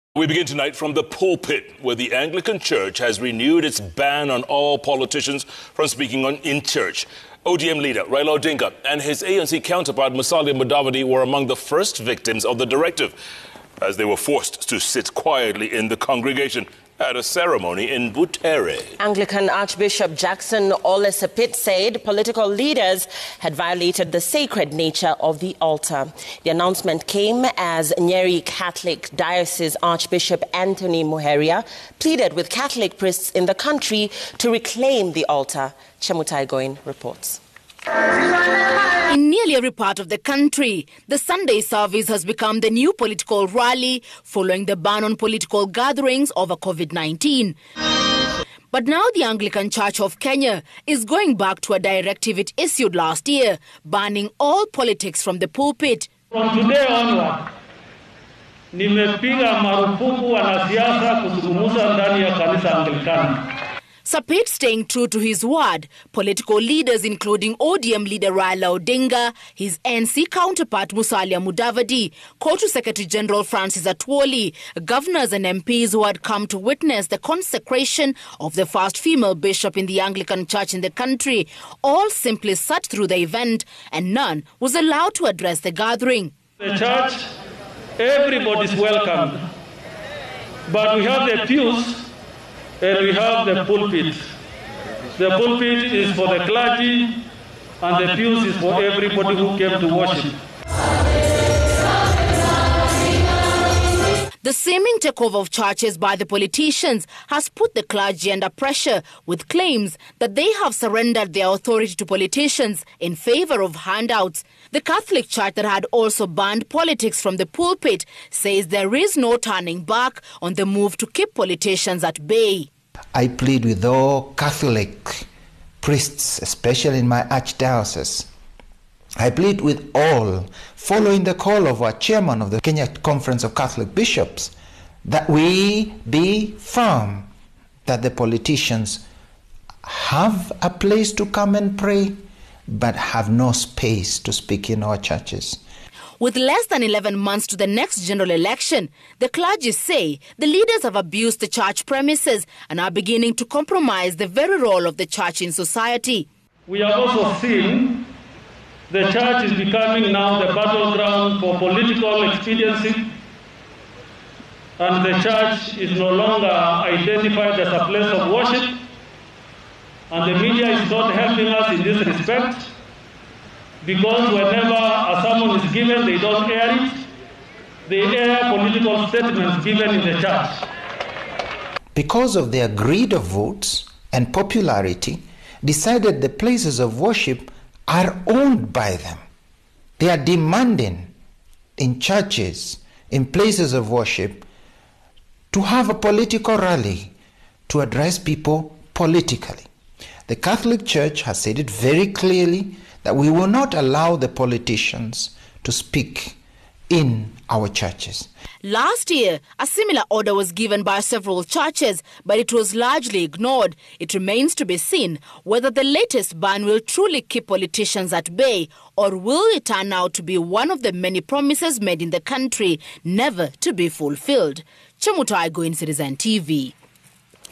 Anglican Archbishop Ole Sapit bans politicians from addressing congregants
The Anglican church has renewed its ban on all politicians from speaking in church. ODM leader Raila Odinga and his ANC counterpart Musalia Mudavadi were among the first victims of the directive as they were forced to sit quietly in the congregation at a ceremony in Butere. Anglican archbishop Jackson ole Sapit said political leaders had violated the sacred nature of the altar. The announcement comes as Nyeri catholic diocese Archbishop Anthony Muheria pleaded with catholic priests in the country, to reclaim the altar.